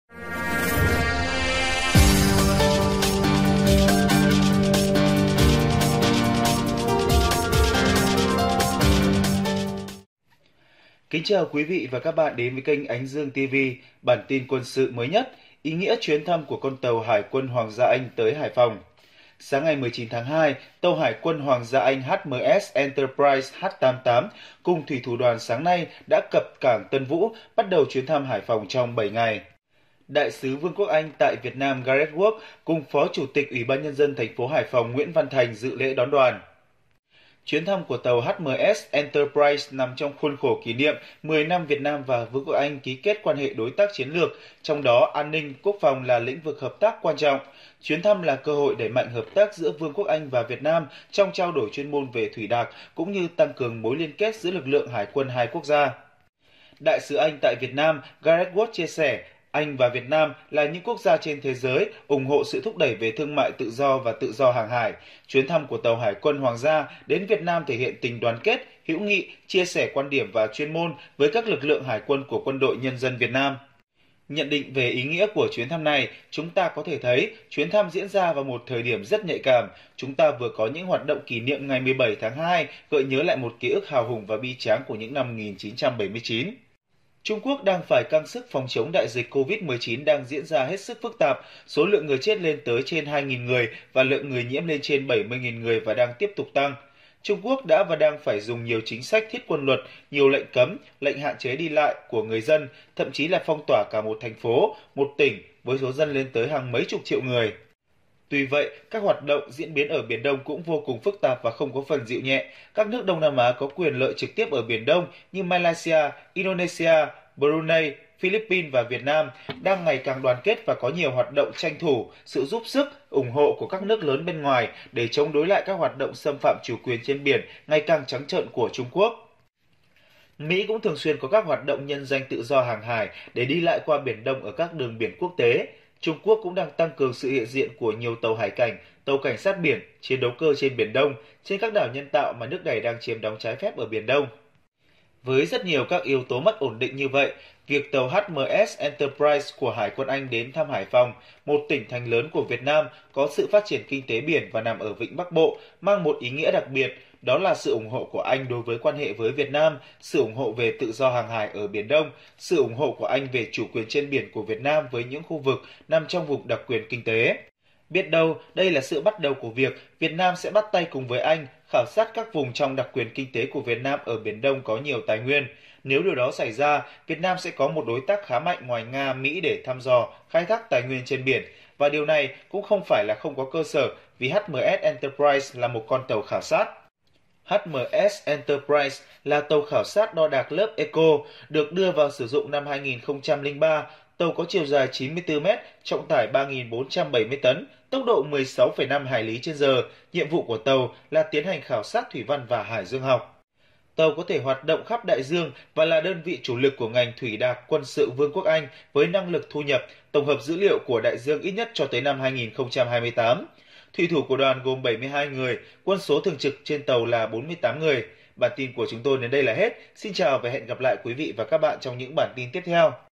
Phía sau chuyến thăm của tàu hải quân Hoàng gia Anh tới Hải Phòng
Sáng ngày 19 tháng 2 Tàu Hải quân Hoàng gia Anh HMS Enterprise (H88) cùng thủy thủ đoàn sáng nay đã cập cảng Tân Vũ, bắt đầu chuyến thăm Hải Phòng trong 7 ngày.
Đại sứ Vương quốc Anh tại Việt Nam Gareth Ward cùng Phó chủ tịch UBND TP Hải Phòng Nguyễn Văn Thành dự lễ đón đoàn.
Chuyến thăm của tàu HMS Enterprise nằm trong khuôn khổ kỷ niệm 10 năm Việt Nam và Vương quốc Anh ký kết quan hệ đối tác chiến lược, trong đó an ninh, quốc phòng là lĩnh vực hợp tác quan trọng. Chuyến thăm là cơ hội đẩy mạnh hợp tác giữa Vương quốc Anh và Việt Nam trong trao đổi chuyên môn về thuỷ đạc cũng như tăng cường mối liên kết giữa lực lượng hải quân hai quốc gia.